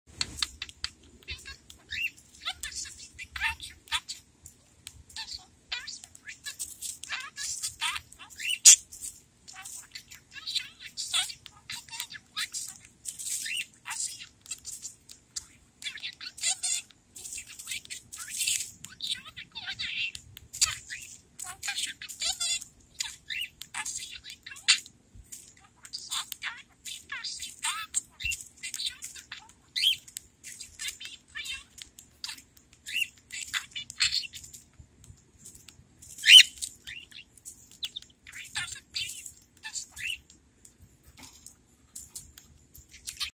"Snuggle Budgie" and the Bell
Skyler Budgie loves to talk to and kiss his toy bell. Short video.